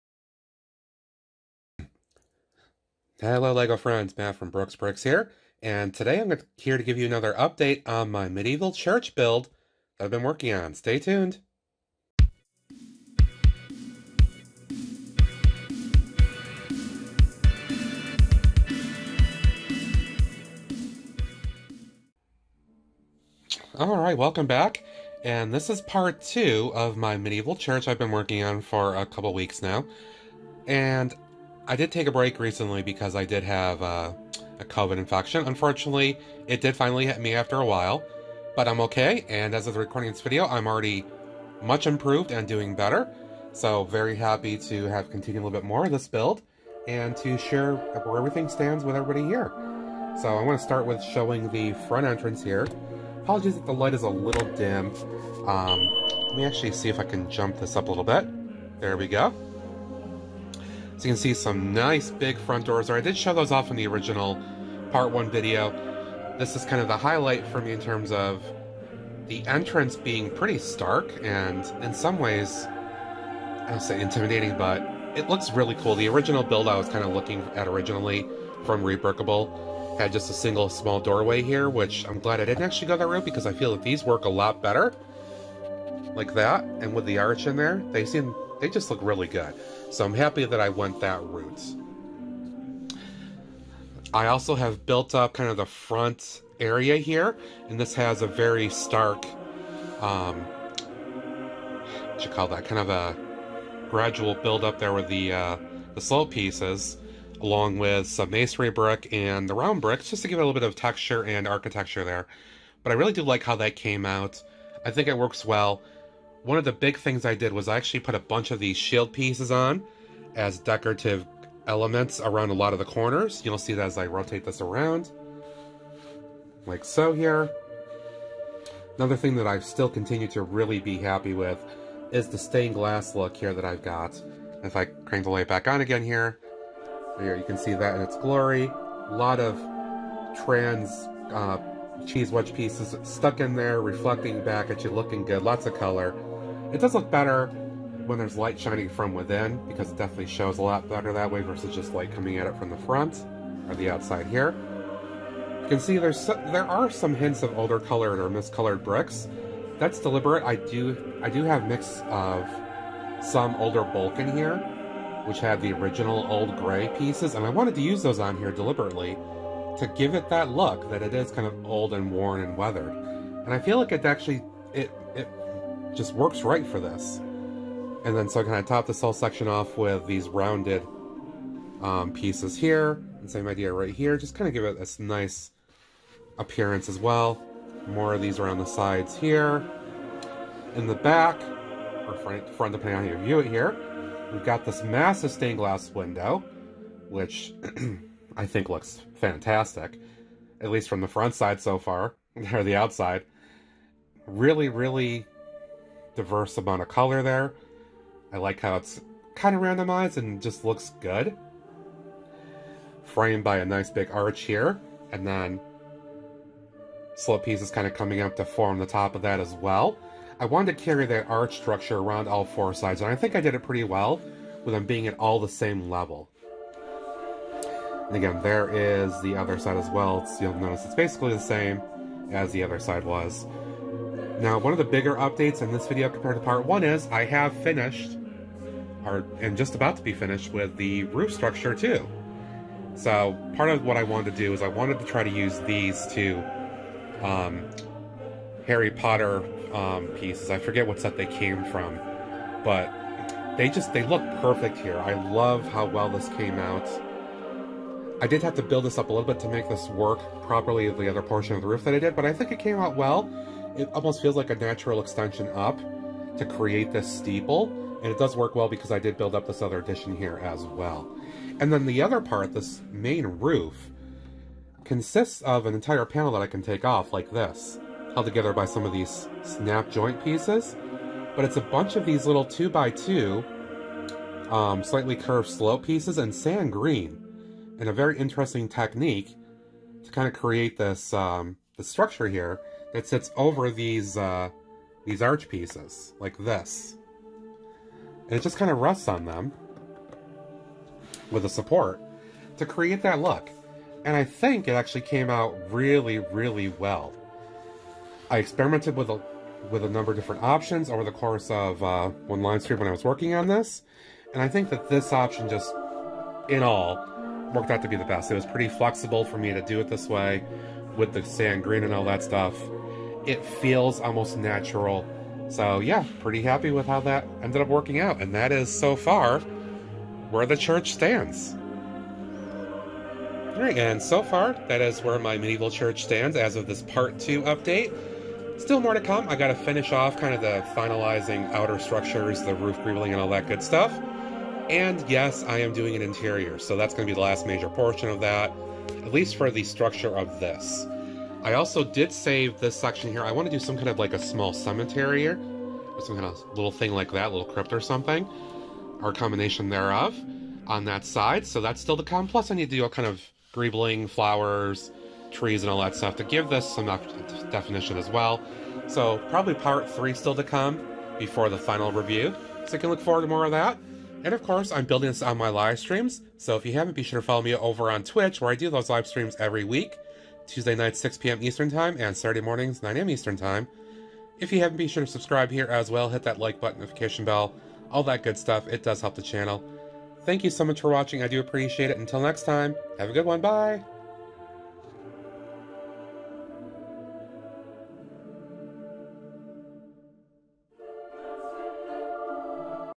Lego MOC Update: Medieval Church ⛪️ | Part 2
A Second Update of my in-progress Medieval MOC Church build!

Catch my Weekly LiveStreams on Twitch - Every Tuesday 6pm and Saturday 9am Eastern!